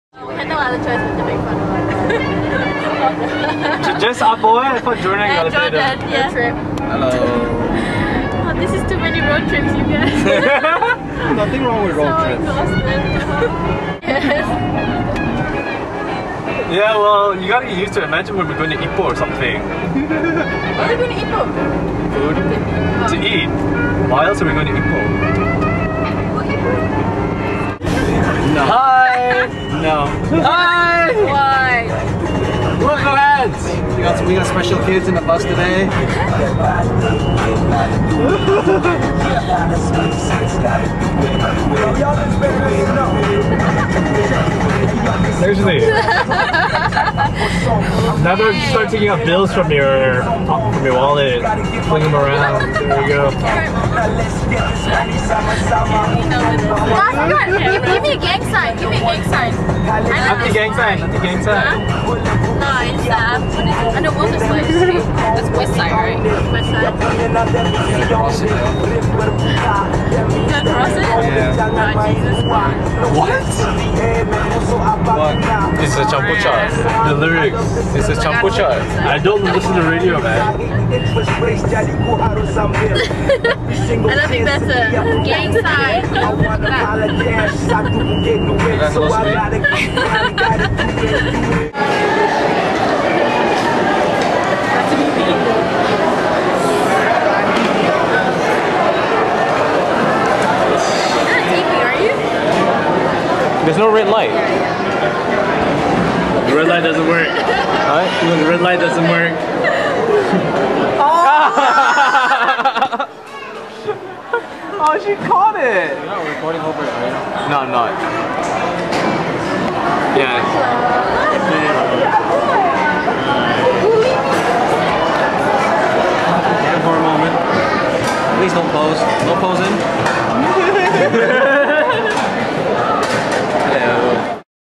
KLSwing on the Road
KLSwing on the way to the shim sham venue. It wasn't so bad, but it did look like it was going to rain.